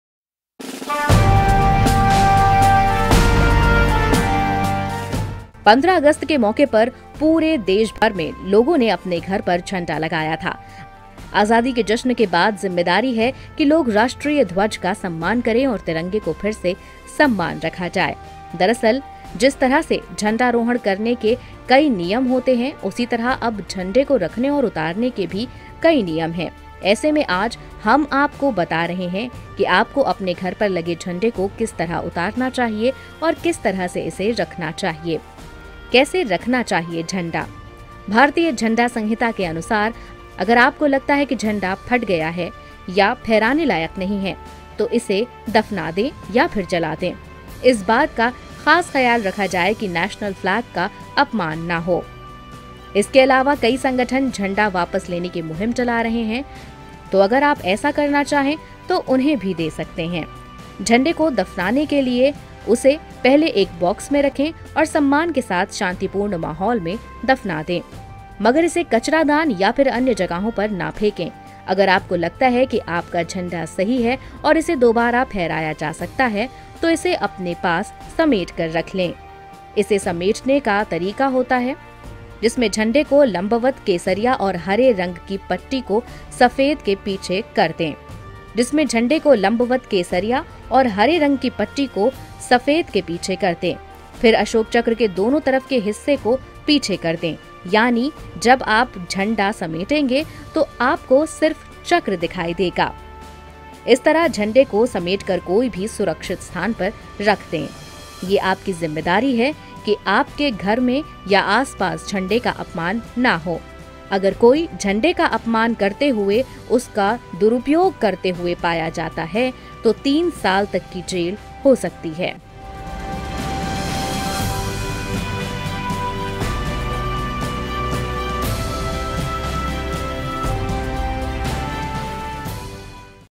क्या आप जानते है तिरंगे को वापस रखने का नियम | Indian Tricolour New Rules !
A Video On The New Rules To Keep In Mind When Hoisting The Indian Tricolour | Har Ghar Tiranga Campaign | Excited About Hoisting Tiranga At Home